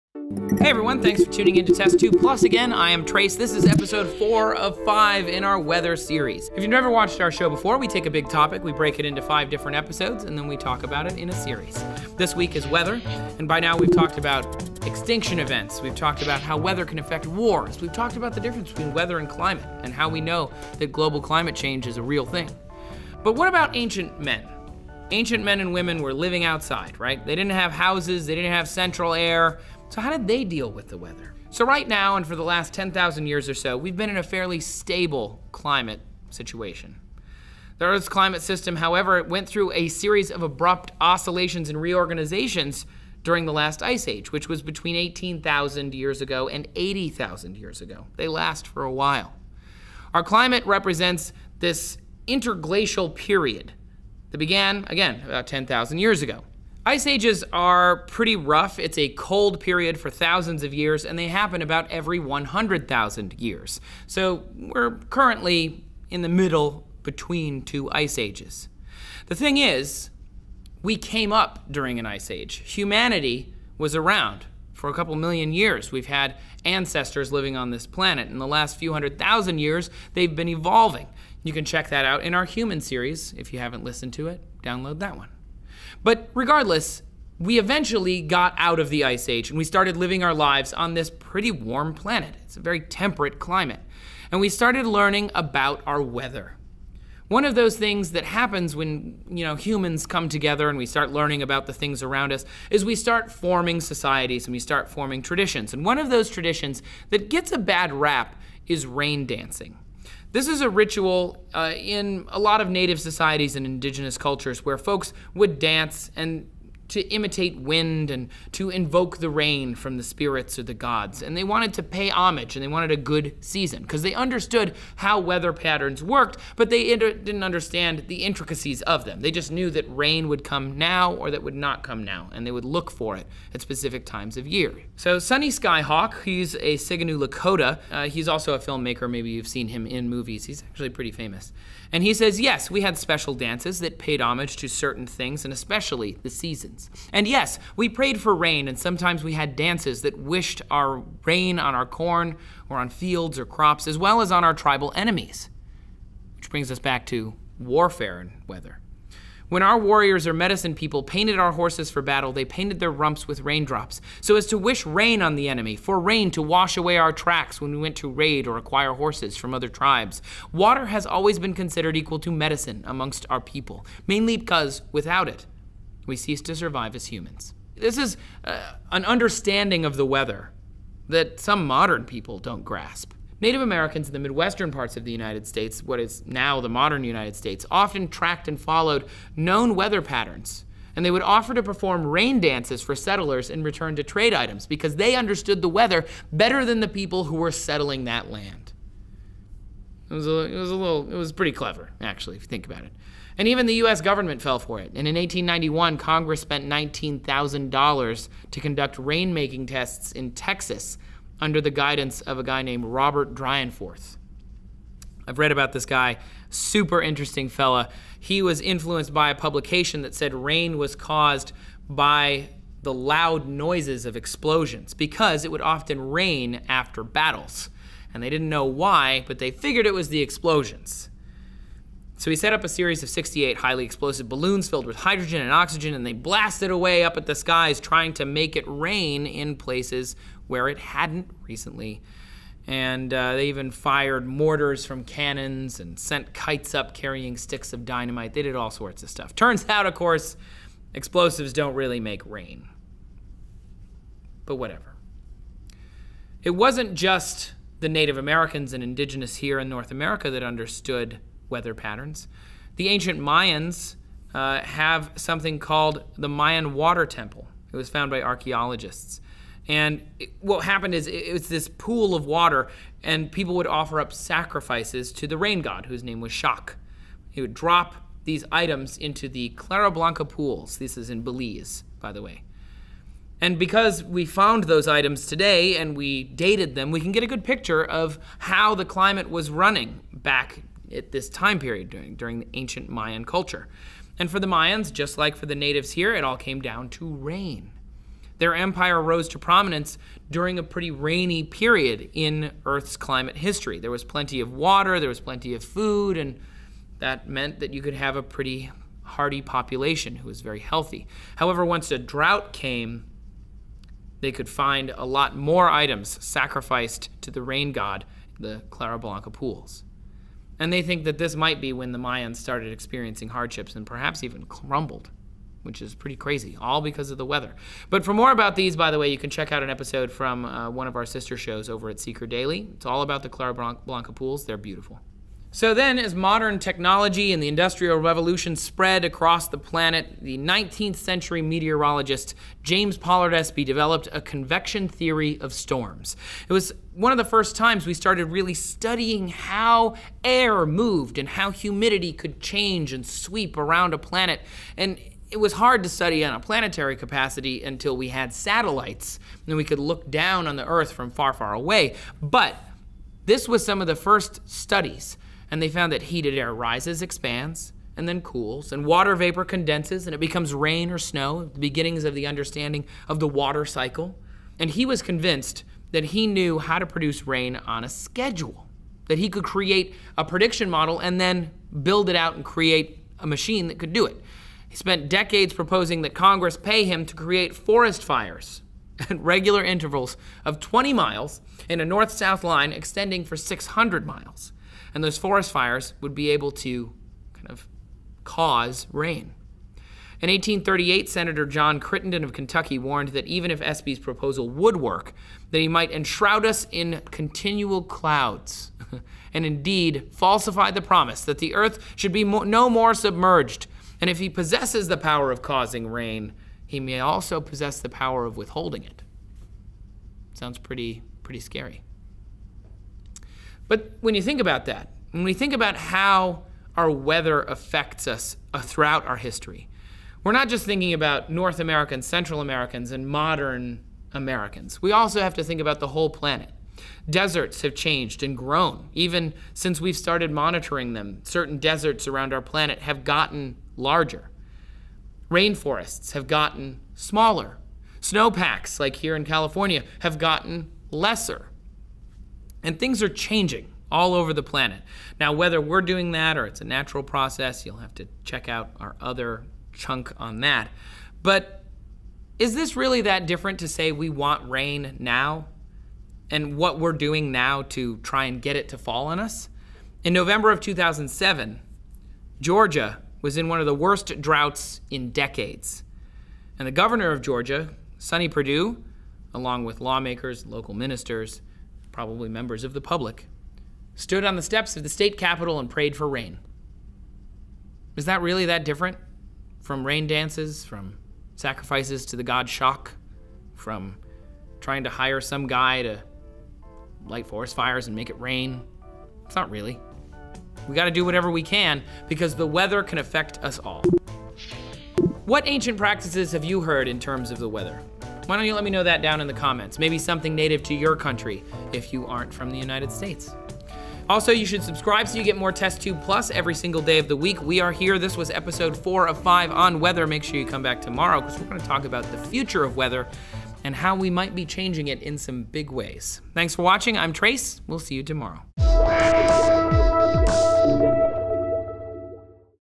How Ancient Man Controlled The Weather
From ancient Mayan sacrifices to Native American rain dancing, humans have been attempting to control the weather for thousands of years. Did any of them succeed?